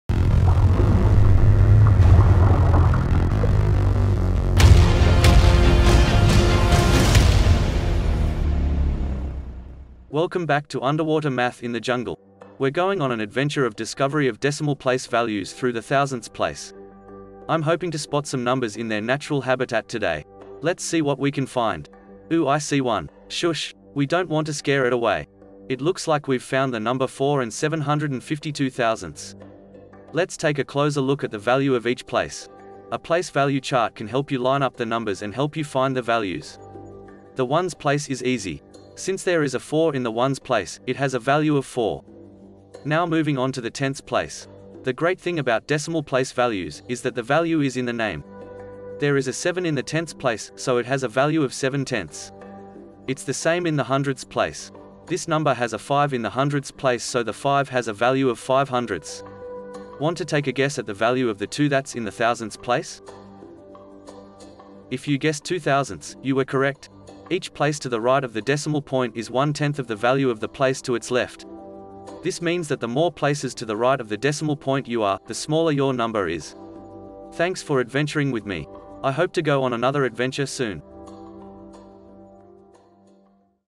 Decimal Place Values Through Thousandths - 5th Grade
Underwater Math provides engaging learning solutions for students. This video helps students represent the value of the digit in decimals through the thousandths. Great for 5th grade math students. TEKS 5.2.A.